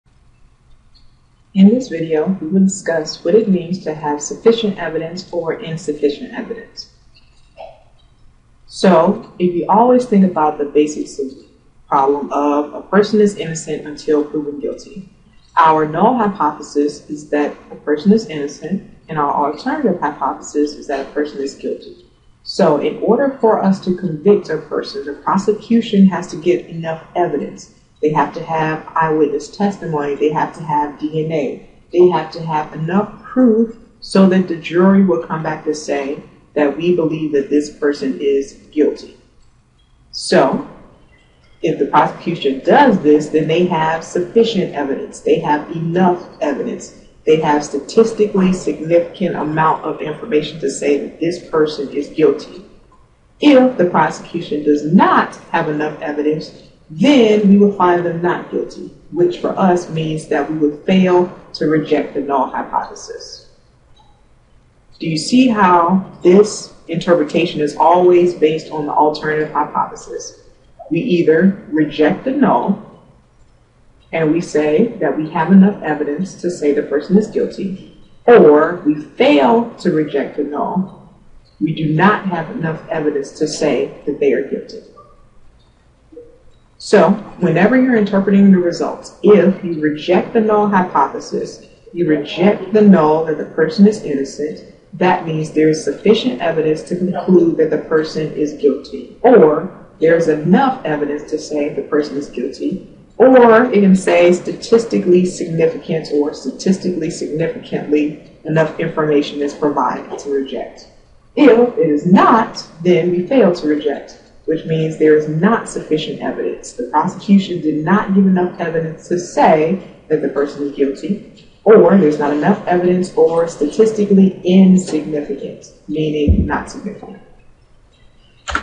Sufficient or INsufficient (How do you interpret your results in Hypothesis Testing?)
**You can change the clarity of the video by changing your settings.  Click on the sprocket "wheel" under the video.**

In this video, you will see how to interpret your results in Hypothesis Testing.